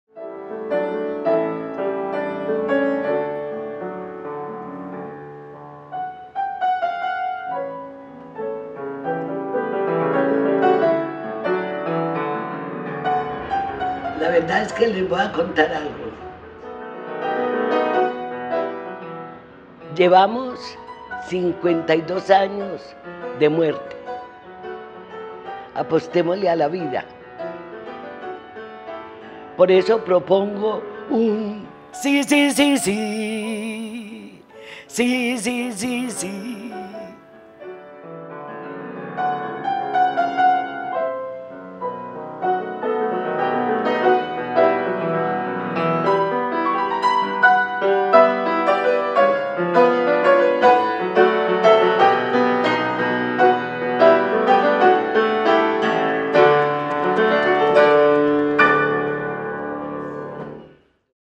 Muchas razones para decir Sí: Teresita Gómez
Existen muchas razones para decir Sí. Estas son las de la pianista Teresita Gómez. Llegó el tiempo de Confiar.